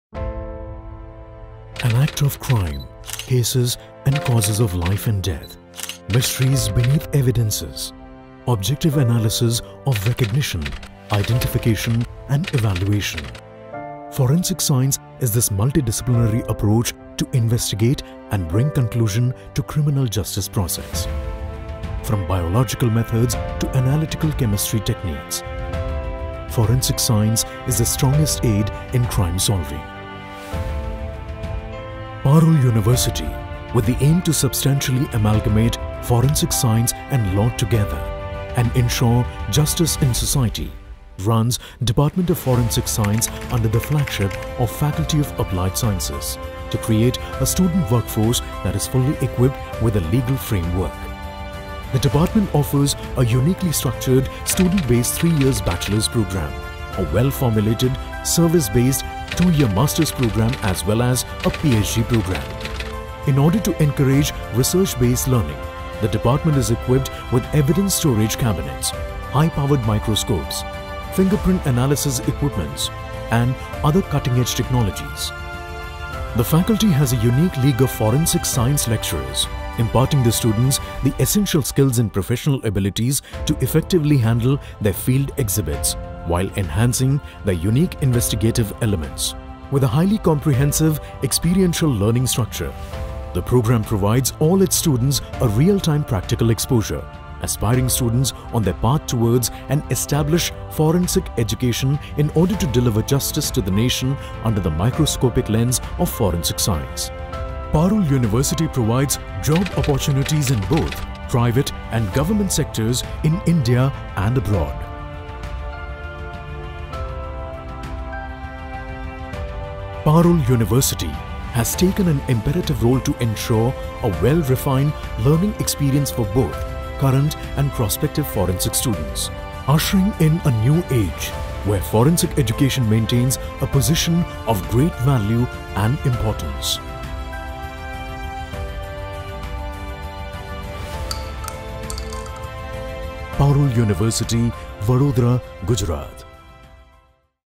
B.Sc | M.Sc | Ph.D in Forensic Science | Parul University
Parul University with the aim to substantially amalgamate Forensic Science and Law together, and ensure justice in society, runs Department of Forensic Science under the flagship of Faculty of Applied Sciences, to create a student workforce that is fully equipped with the legal framework needed for the admission of evidence in the courts along with other relevant law enforcement agencies. The department offers a uniquely structured student based three year bachelors program along with a well formulated, service based, Two year masters program and a Ph.D Programme.